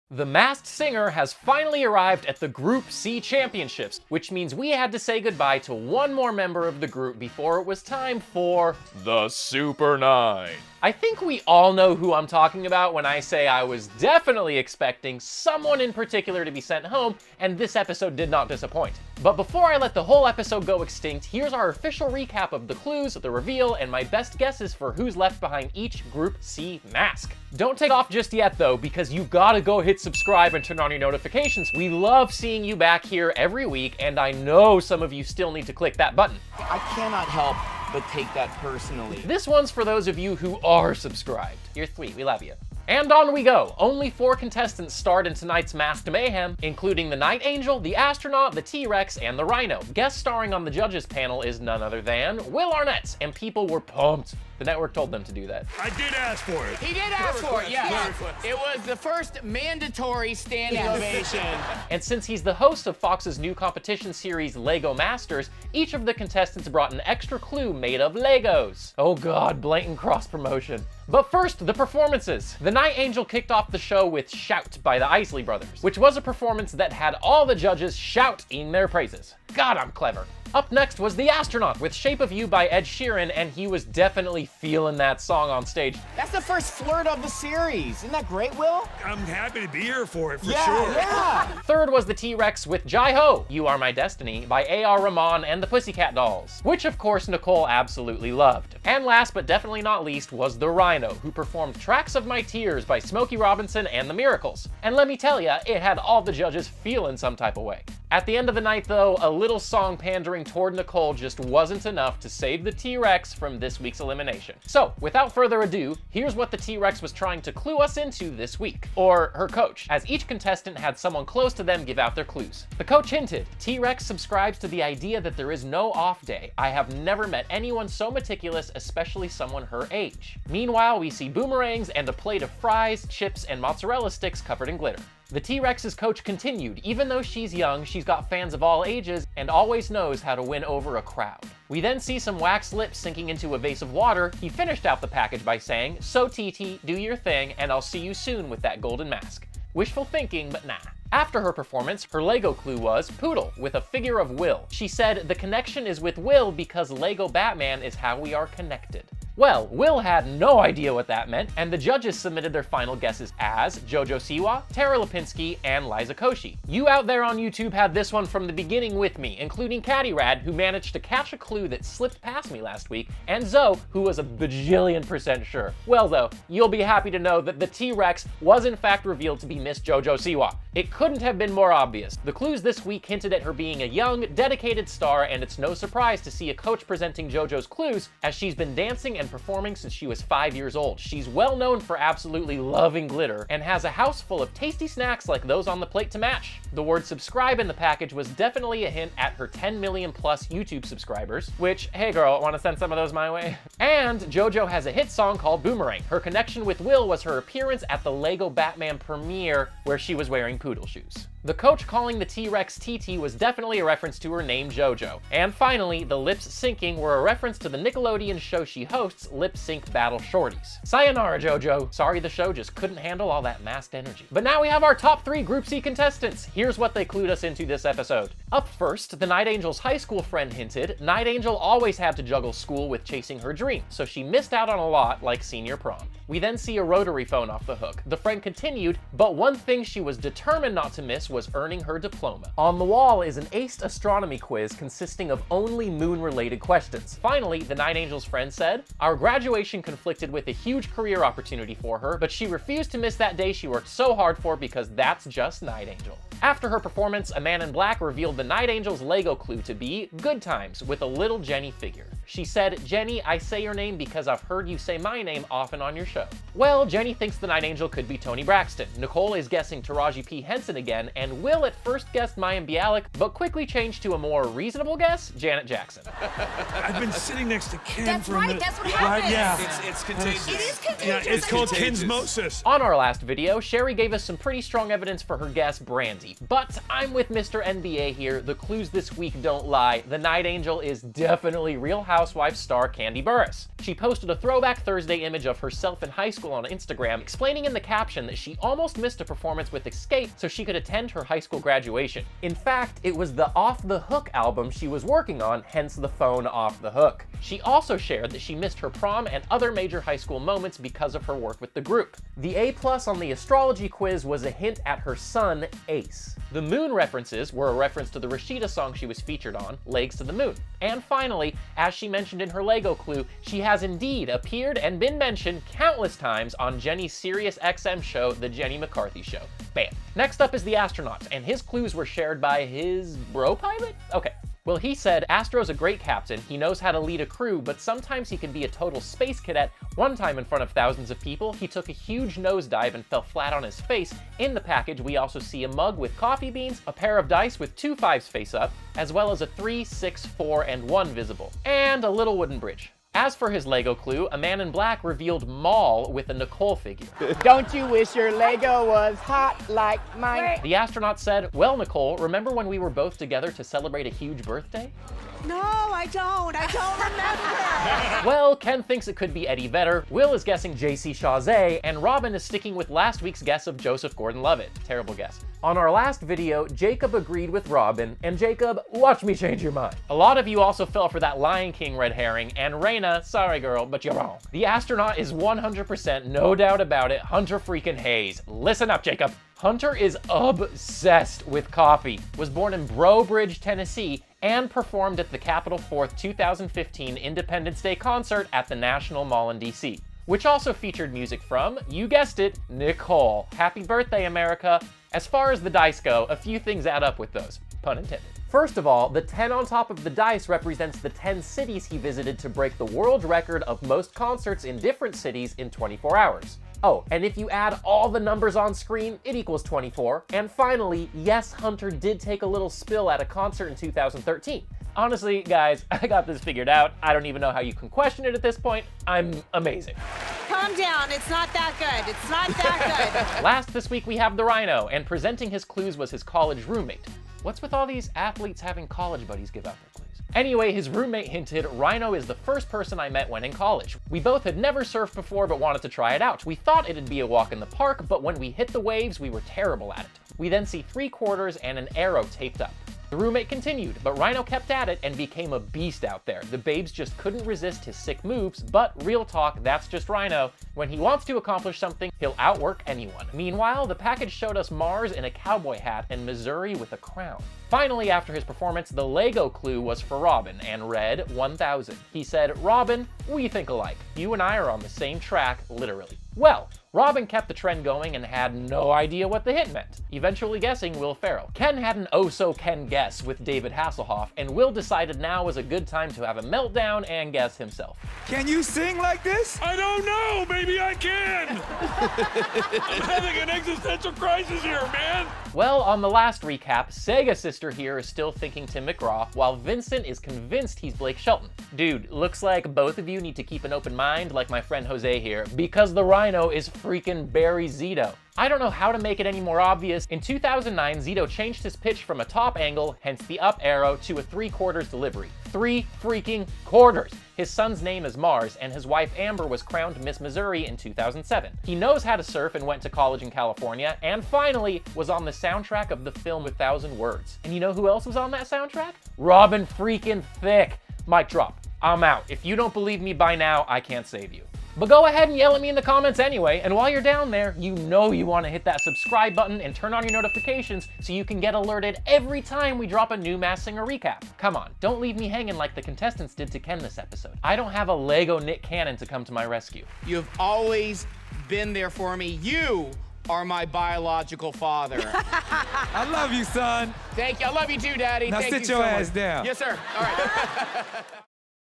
The Masked Singer Season 3 | Episode 9 Spoilers, Clues and Guesses
The Masked Singer has captivated America, and it isn’t hard to see why. The elaborate costumes, the performances, and the big mystery behind the identities of its celebrity competitors have America invested and committed to finding out who each contestant really is. The Masked Singer Season 3, Episode 9, featured the remaining Group C singers (Night Angel, Astronaut, T-Rex and Rhino). Here’s our best guesses for the remaining contestants, plus our recap of the performances and the reveal!